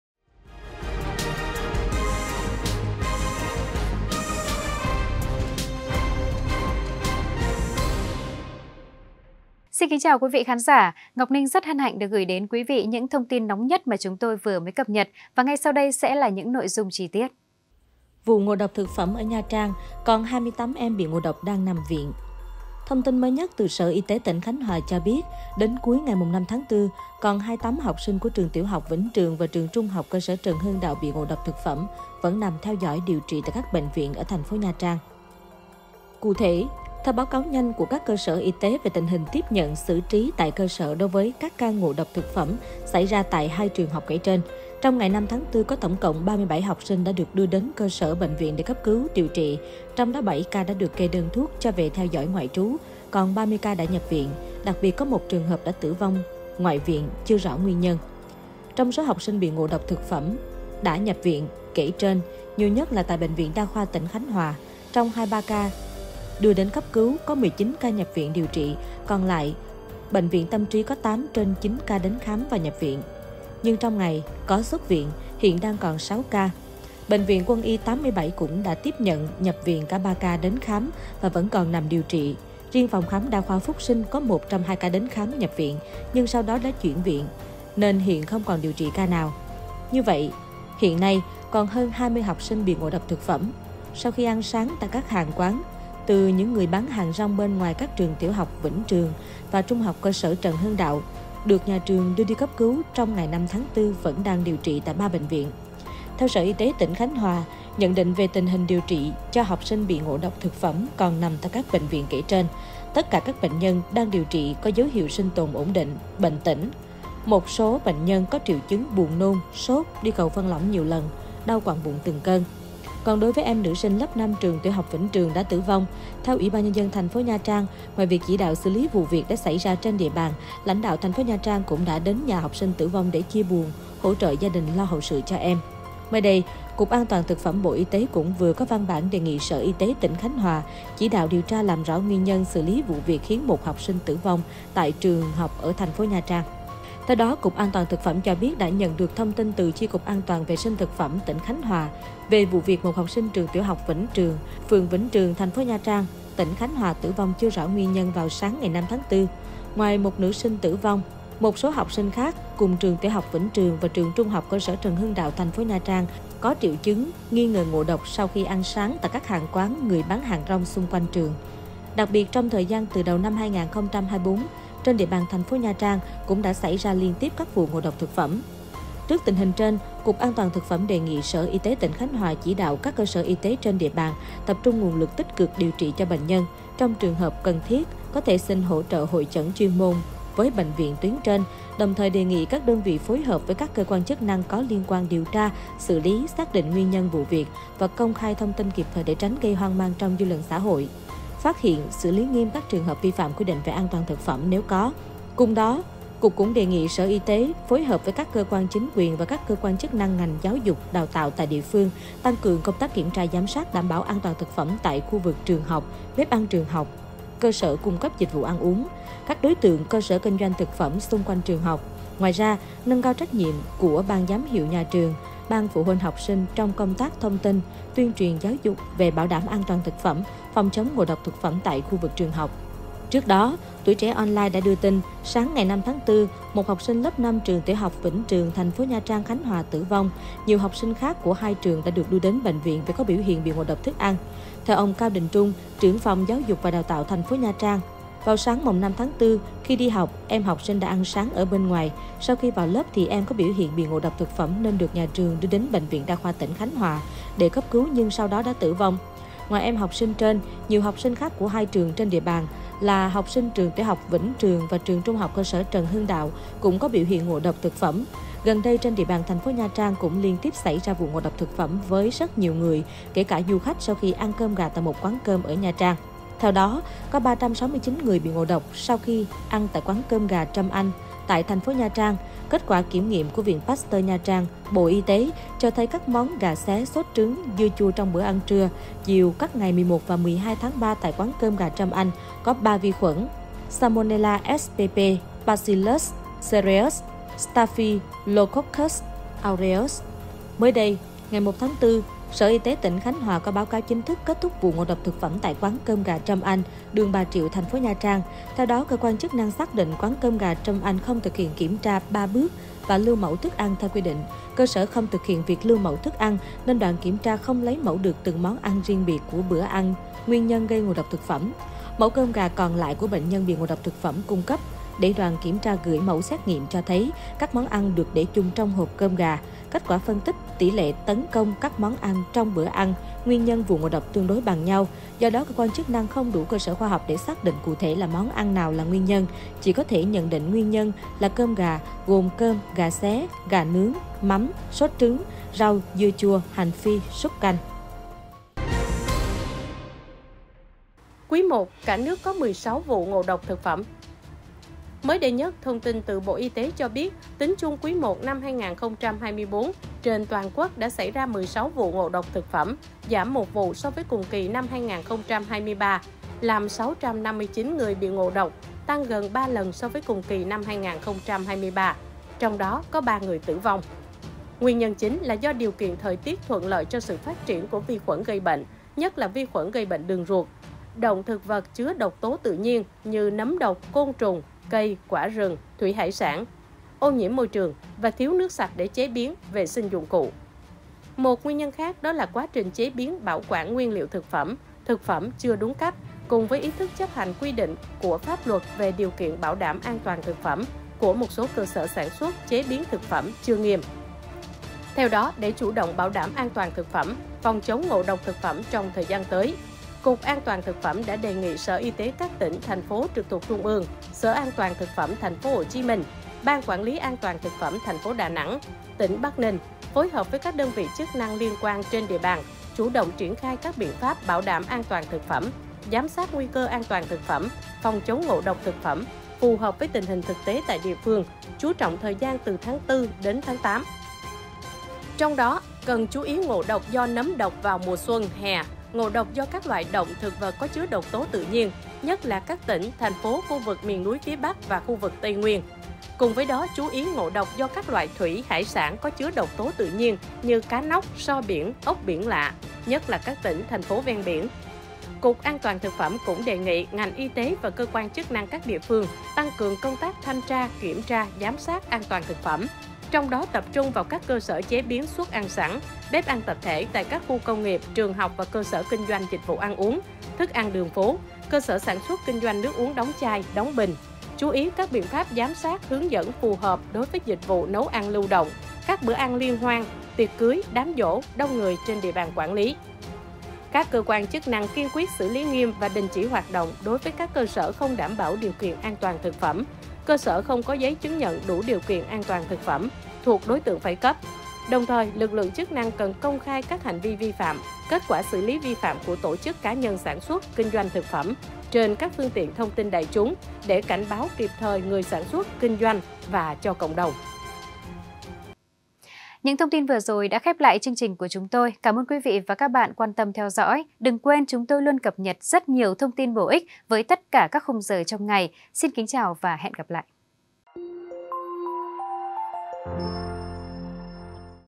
Sức khỏe 28 học sinh ngộ độc thực phẩm ở Nha Trang chưa tiến triển tốt để có thể ra viện | CafeLand
CafeLand | Thông tin mới nhất từ Sở Y tế tỉnh Khánh Hòa cho biết, đến cuối ngày 5-4, còn 28 học sinh của Trường tiểu học Vĩnh Trường và Trường THCS Trần Hưng Đạo bị ngộ độc thực phẩm vẫn nằm theo dõi, điều trị tại các bệnh viện ở TP Nha Trang. Theo Sở Y tế tỉnh Khánh Hòa, nhận định về tình hình điều trị cho học sinh bị ngộ độc thực phẩm còn nằm tại các bệnh viện kể trên: "Tất cả các bệnh nhân đang điều trị hiện dấu hiệu sinh tồn ổn định, bệnh tĩnh. Một số bệnh nhân có triệu chứng buồn nôn, sốt, đi cầu phân lỏng nhiều lần, đau quặn bụng từng cơn". Còn đối với em nữ sinh lớp 5 Trường tiểu học Vĩnh Trường đã tử vong, theo UBND TP Nha Trang, ngoài việc chỉ đạo xử lý vụ việc đã xảy ra trên địa bàn, lãnh đạo TP Nha Trang cũng đã đến nhà học sinh tử vong để chia buồn, hỗ trợ gia đình lo hậu sự cho em.
Mới đây, Cục An toàn thực phẩm (Bộ Y tế) cũng vừa có văn bản đề nghị Sở Y tế tỉnh Khánh Hòa chỉ đạo điều tra làm rõ nguyên nhân, báo cáo rõ về vụ việc kể trên. Đặc biệt là khi trong thời gian từ đầu năm 2024, trên địa bàn TP Nha Trang cũng đã xảy ra liên tiếp các vụ ngộ độc thực phẩm.